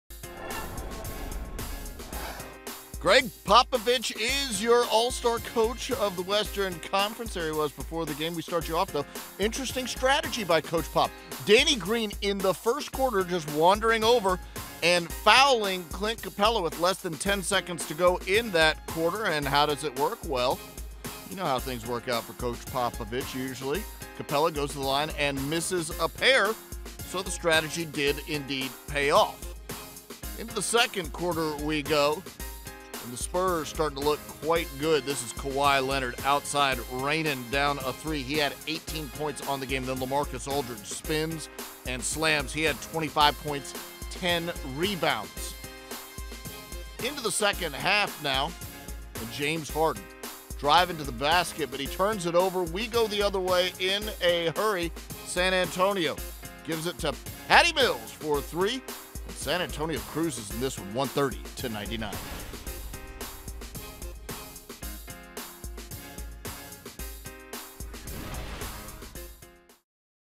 Houston Rockets vs San Antonio Spurs - January 27, 2016
LaMarcus Aldridge scores 25 points with 10 rebounds to lead San Antonio over Houston, 130-99.